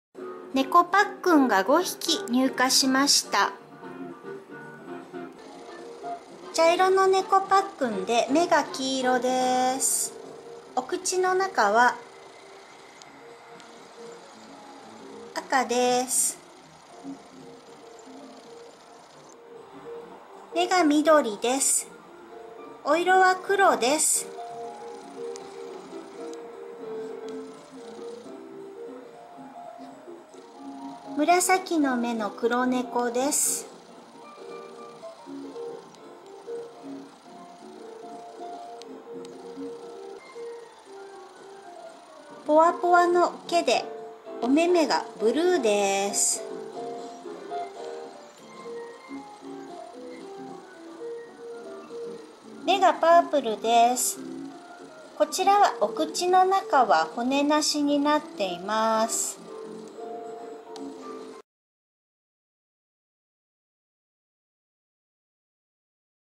きゃめさん(神奈川県)｜2013.8.24入荷情報 ギャラリーのろぺこ
ネコパックン、牛柄のコ(頭が開くタイプ)1点になっていましたが、今日5匹追加になりました。

今回は4匹は口の中に骨があるバージョン。 1匹は骨なしバージョンです。

目の色、そして口の中の色が1つ1つ違います。

白系のネコちゃんは1匹はポワッとした毛です。

ほぼ毎日全国から新作が入荷します♪　（お店の休日以外は毎日情報を更新しています。）

こちらでご紹介の動画作品は下記の『新入荷情報ページ』より作家さんの作品通販のページにも飛べますが、ハンドメイドなのでお嫁入りの場合はごめんなさい！